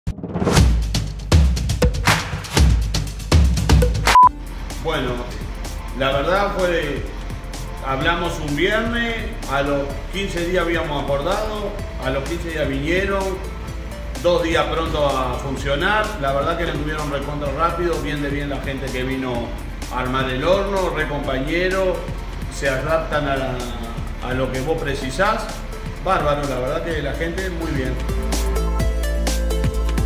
Moises Perez, compra y puesta en marcha.
Testimonio sobre el proceso de compra y puesta en marcha del horno ARGENTAL FE-III 960 Biopellet, por Moises Pérez.